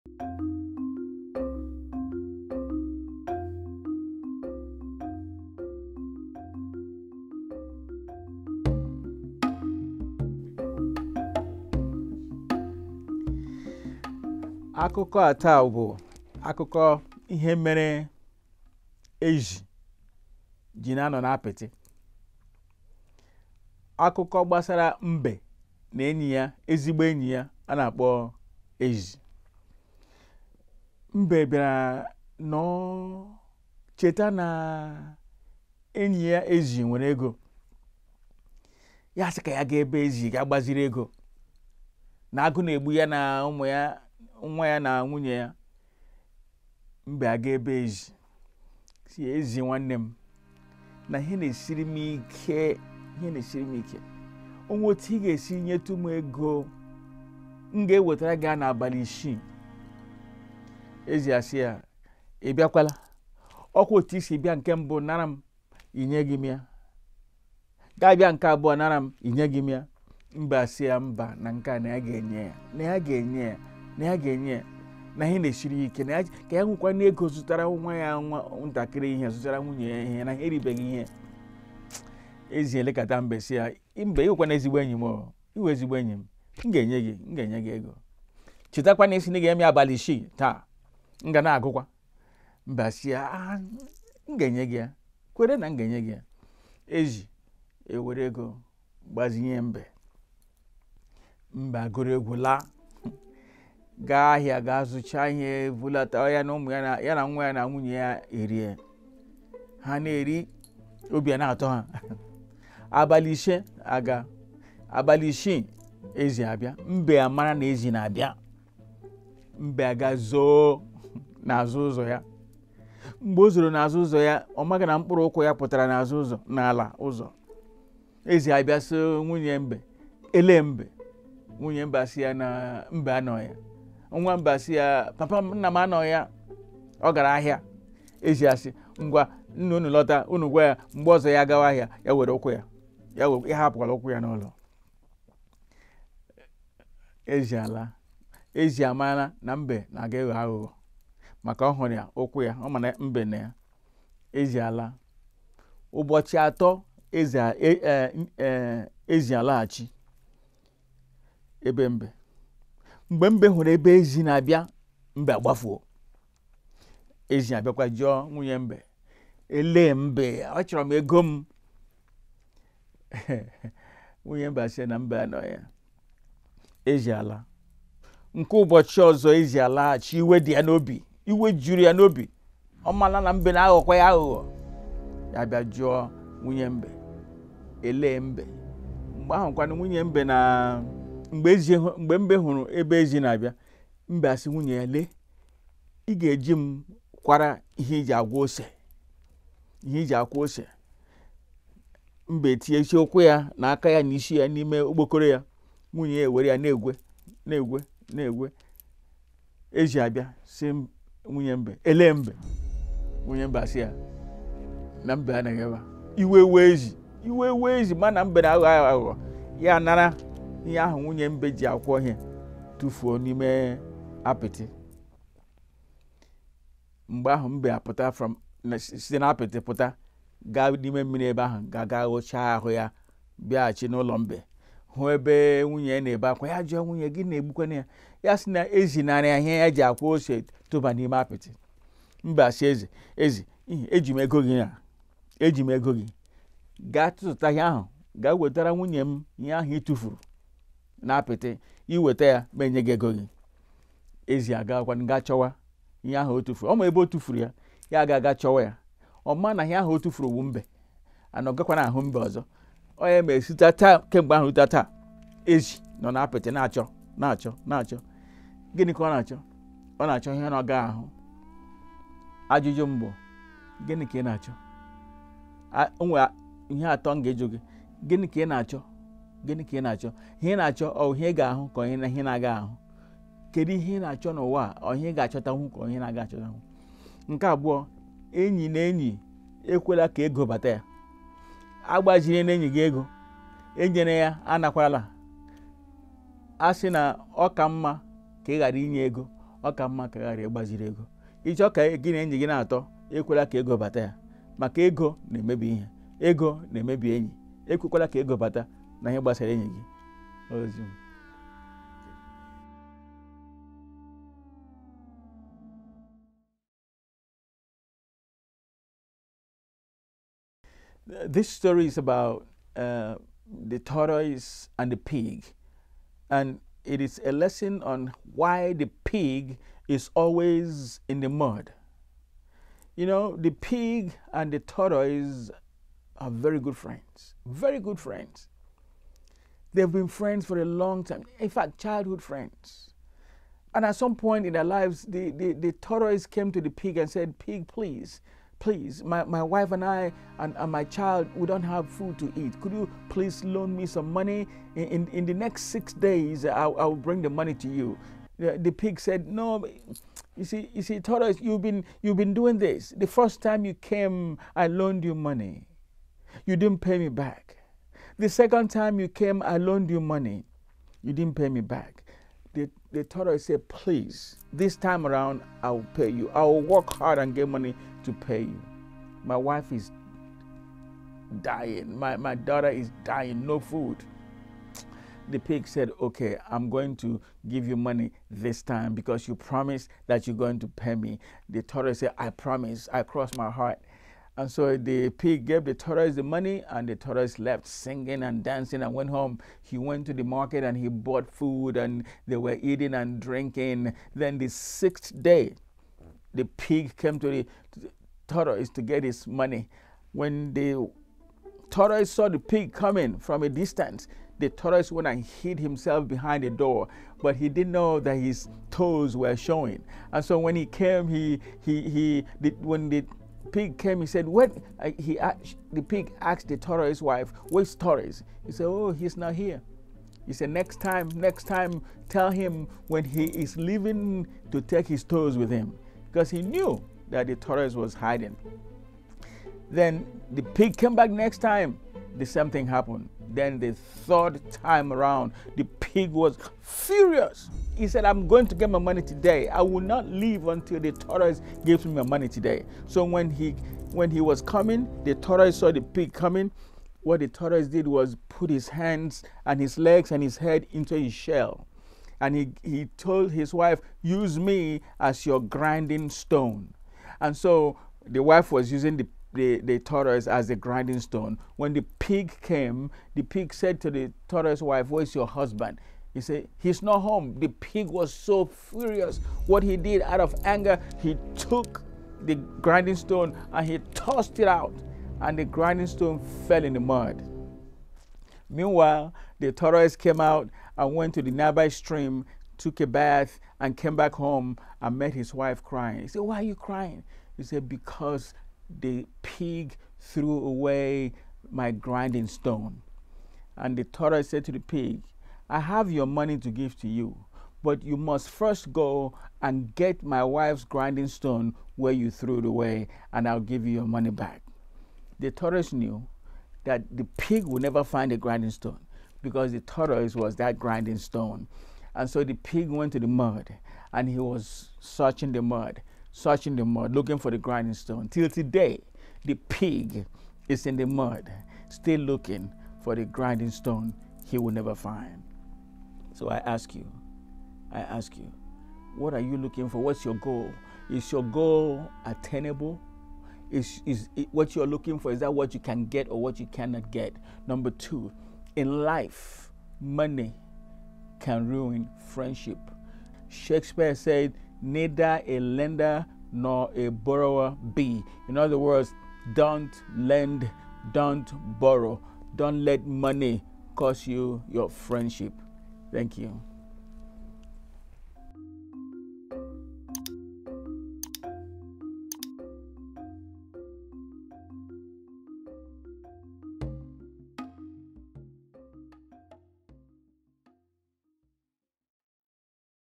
Why the Pig is always in the Mud | A Nigerian folk tale narrated by Rev. Dr Darlington Ndubuike
Dive into the rich tapestry of African folklore with this captivating video featuring the unforgettable tale of the Pig and Tortoise. Embark on a mesmerizing journey through the enchanting landscapes of Africa as we bring you a timeless story steeped in culture, wisdom, and adventure. | An Igbo tale for adults and children

0:00 - Intro
0:14 - Tale in Igbo
05:15- Tale in English